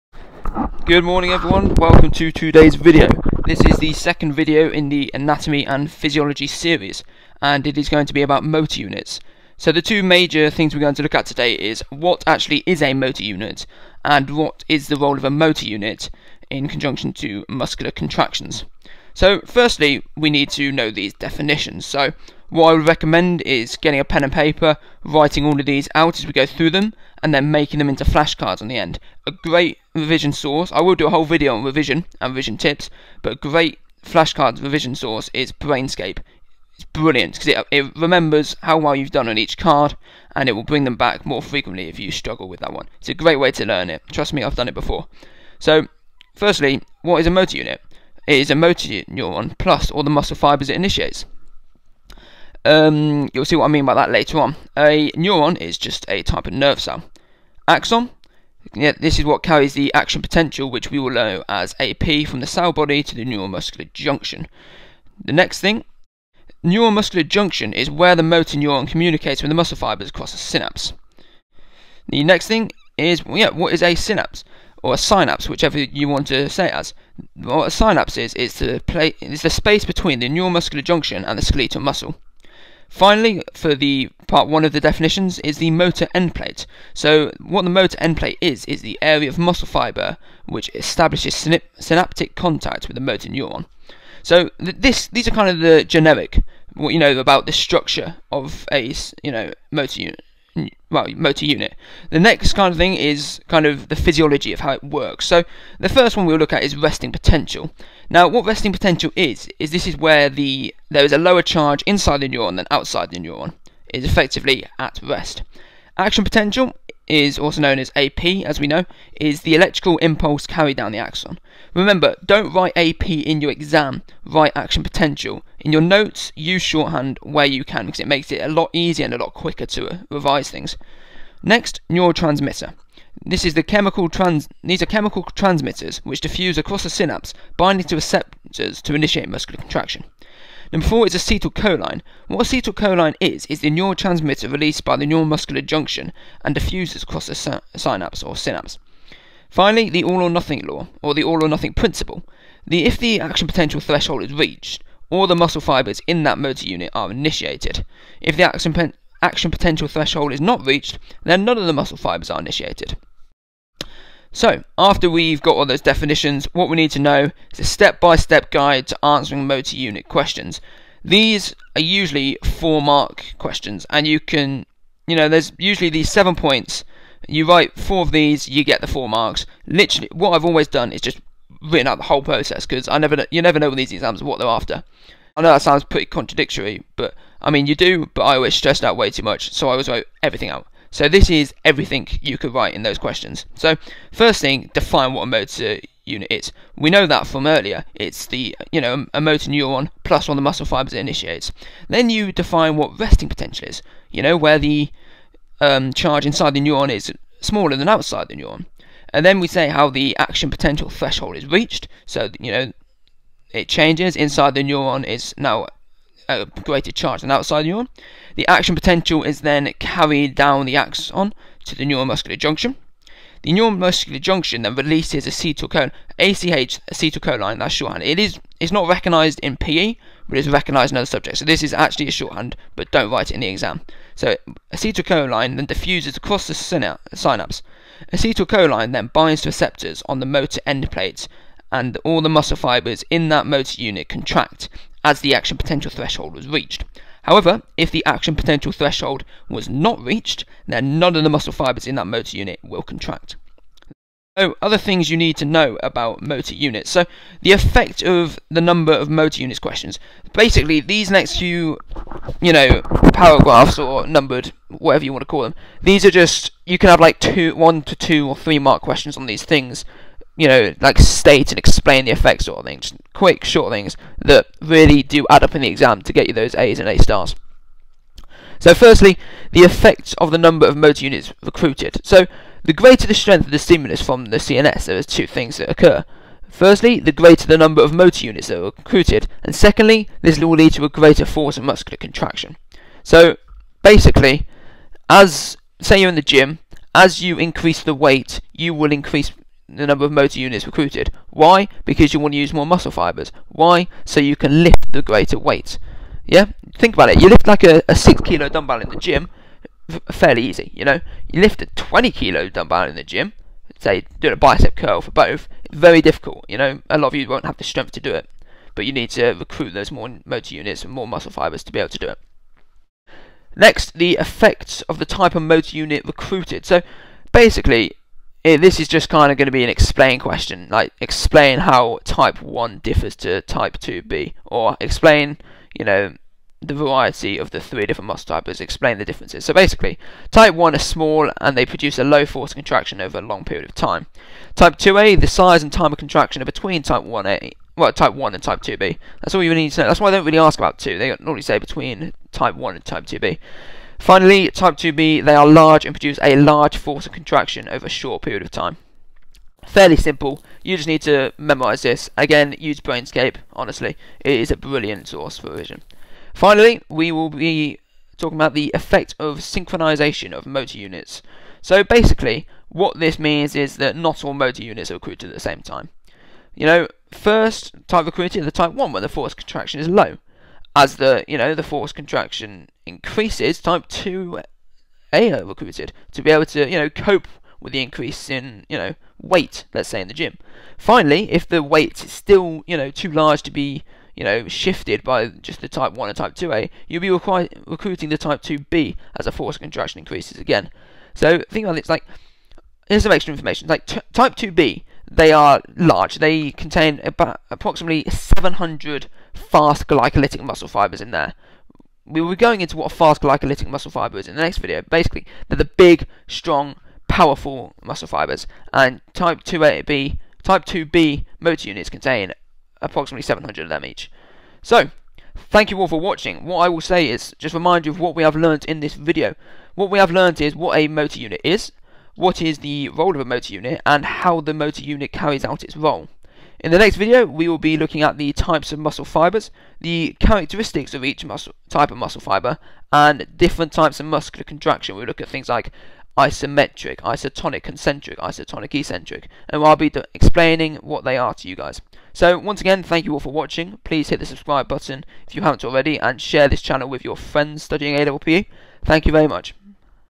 OCR A Level PE - Motor Units
In this video we look at the anatomy of a motor unit and the physiology behind how the motor unit contributes to muscular contractions